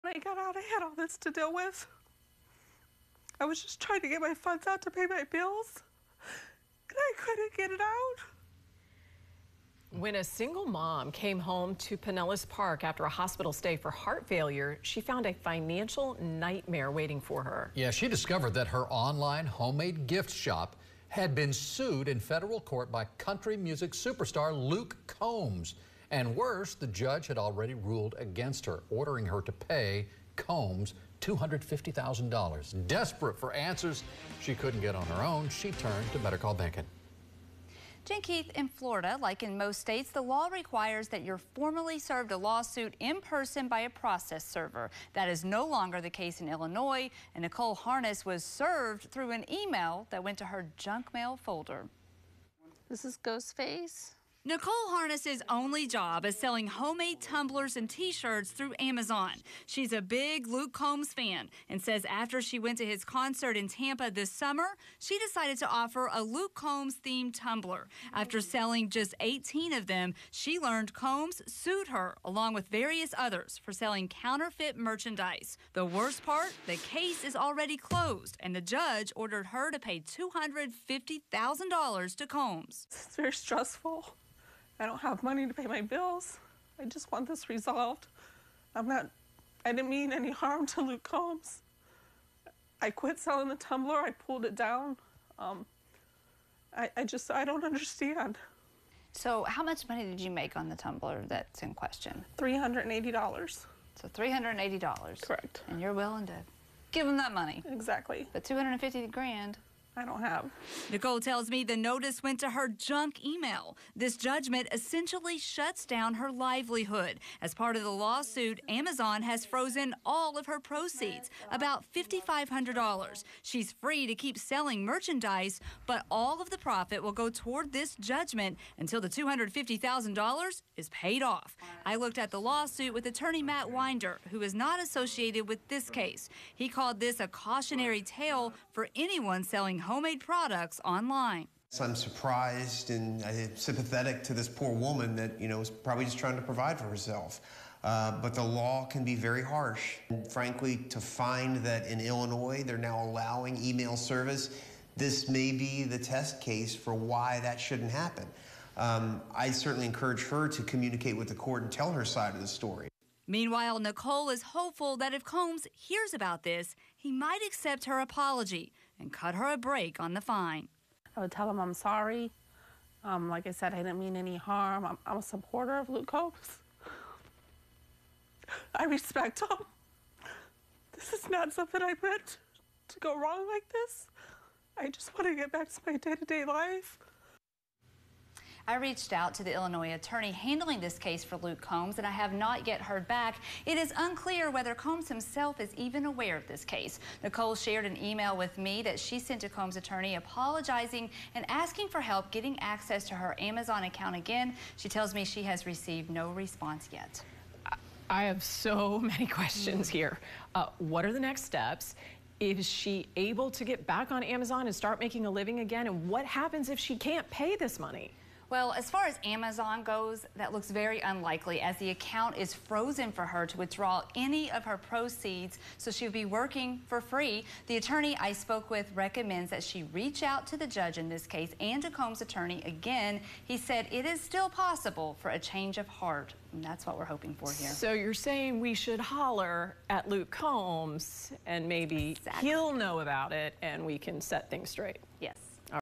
Pinellas Park woman sold 18 Luke Combs- themed tumblers, now owes the star $250,000 in federal lawsu
Pinellas Park woman sold 18 Luke Combs- themed tumblers, now owes the star $250,000 in federal lawsuit